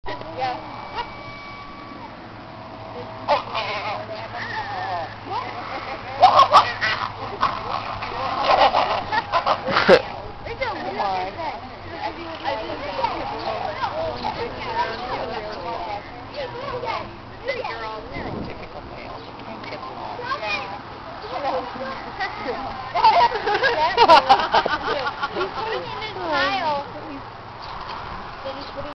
Wolf Pups Displaying Dominance for Food Dish. (Not Michael Vick Filming)
A video I filmed on a family vacation quite a long time ago.  I love wolves.